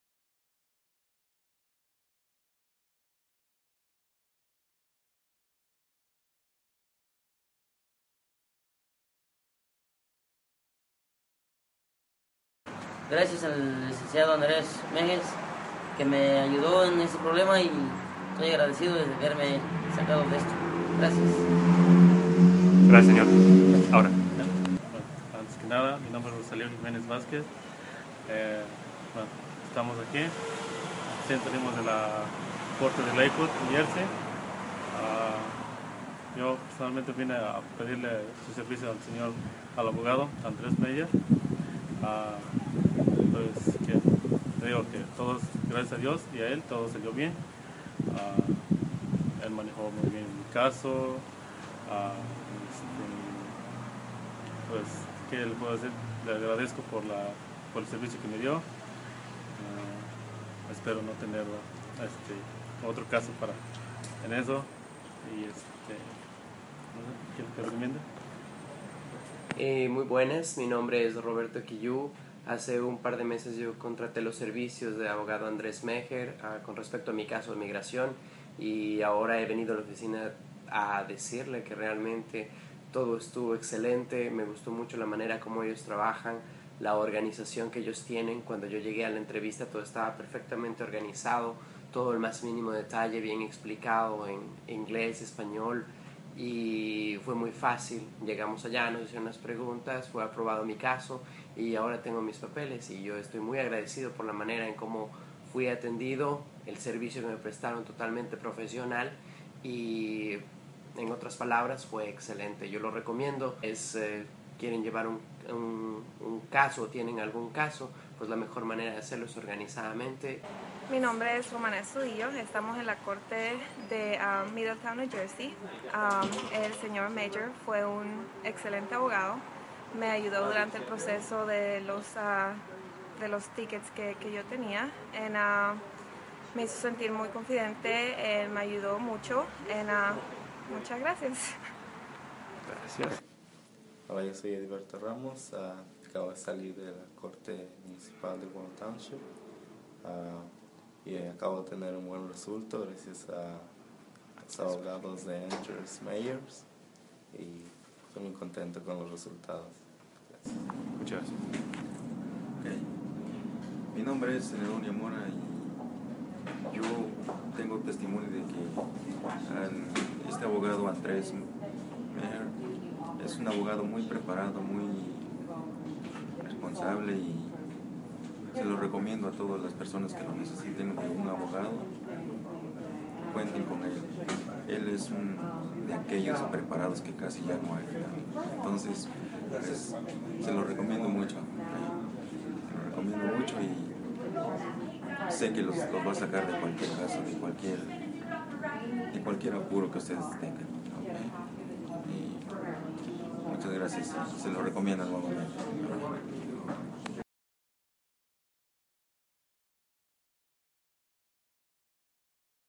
New Jersey Attorney Andres Mejer's Client Testimonials | New Jersey Immigration Attorney
| In this video clients give their own personal experience of hiring Andres Mejers.

Contact Andres Mejer Law today at